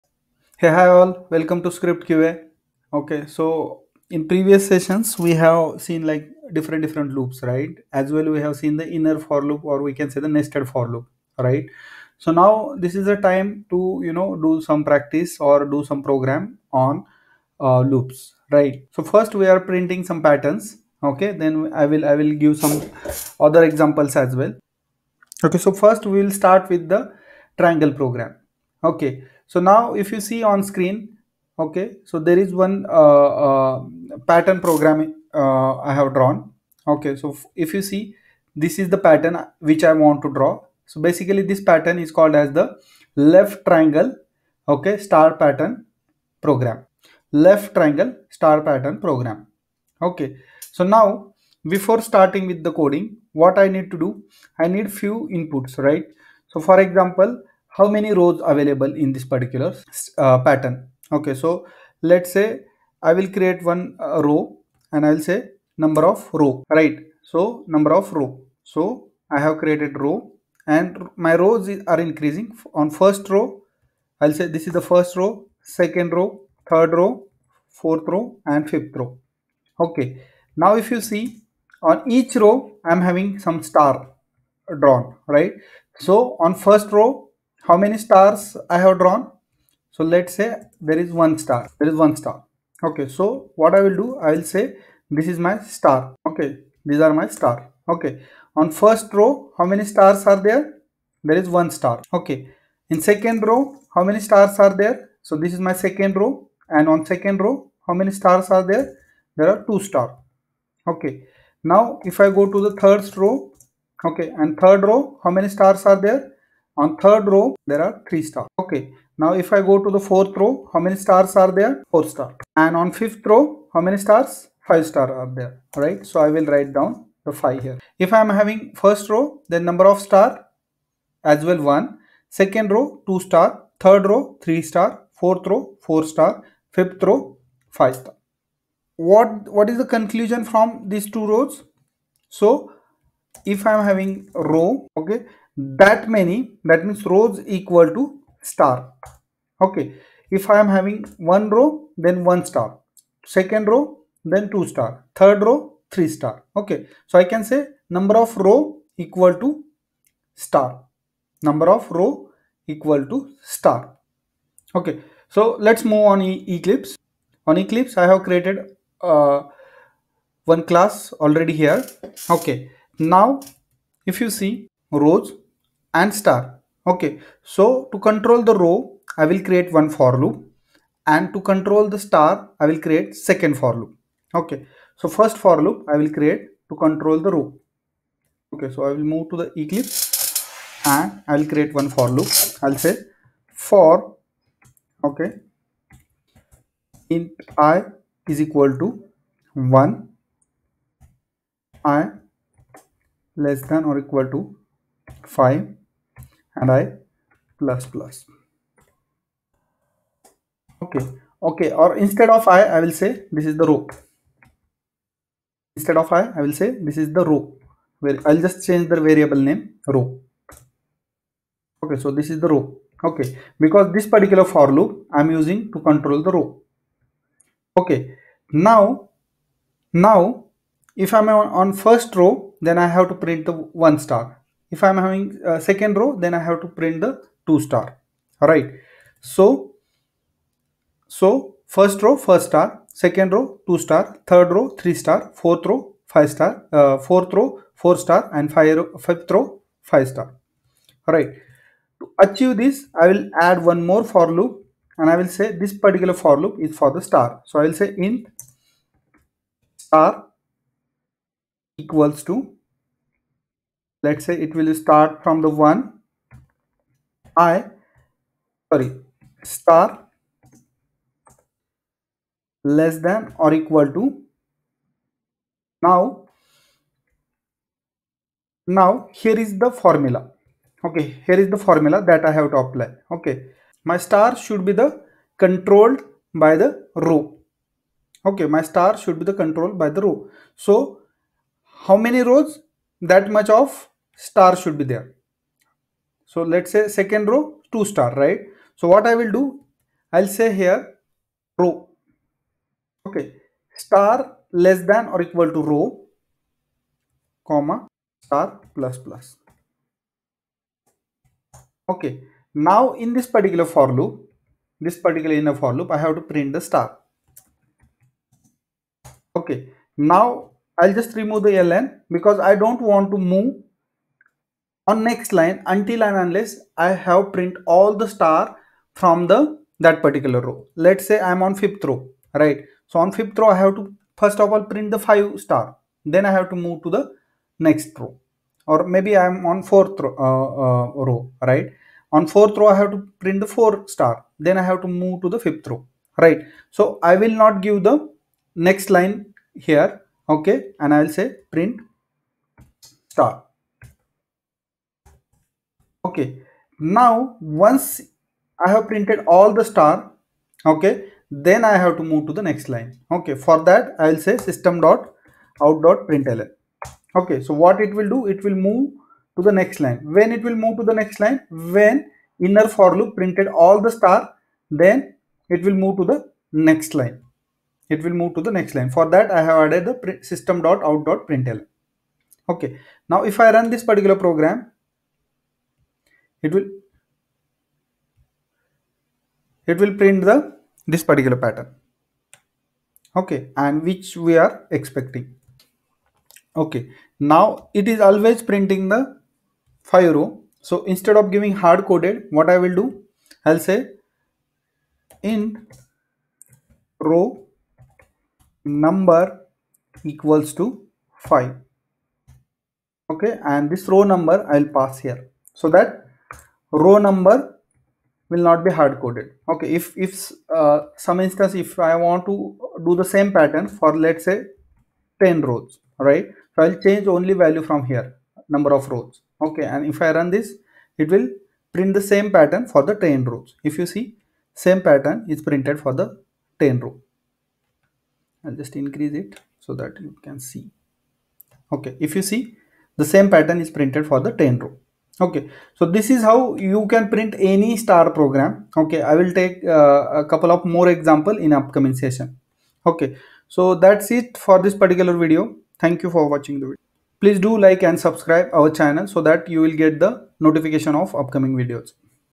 Program 1: Java Programming Tutorial: Print Left Triangle Pattern
In this Java programming tutorial, you will learn how to write a program to print a left triangle pattern using asterisks. A left triangle pattern is a pattern of asterisks that forms a triangle, with the left side of the triangle being the longest side.

The tutorial will cover the logic behind the program, which involves using nested loops and conditional statements. It will explain each step of the code in detail, making it easy for beginners to follow along.

Throughout the tutorial, you will learn important programming concepts such as loops and conditional statements in Java. These are fundamental concepts that are essential for any Java programmer to master.

By the end of the tutorial, you will have a solid understanding of how to print a left triangle pattern in Java and will be equipped with the knowledge to tackle more complex programming challenges.

how to print a star patterns in java using eclips, java patterns.
java, programming, tutorial, print, left triangle pattern.